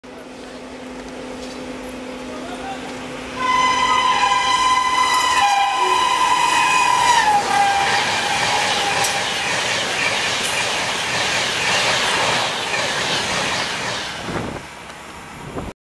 INDIAN RAILWAYS INDIA'S FIRST CAPTURED HIGH SPEED TRIAL TRAIN DOES 160 KMPH AT ASAOTI.
GZB WAP-5 30013 Nakirti Hauling 12 LHB Coaches (2 First AC Chair Car, 8 AC Chair Car and 2 EOGs). Rushesh past Asaoti at 11:50 AM on 03/07.2014. This train had departed New Delhi at 11:15 AM.

More on this can be read here on these links,

The run details are as follows of the 160kmph trial run between NDLS-AGC-NDLS. 12 LHB bogies

NDLS-AGC (197kms)

Dep NDLS : 11:15
Arr AGC : 12:58

160kmph : 31 minutes 55 seconds
150kmph : 44 minutes 48 seconds

Return journey

AGC departure : 14:57
NDLS arrival : 16:42

160kmph : 28 minutes 34 seconds
150kmph : 37 minutes 53 seconds.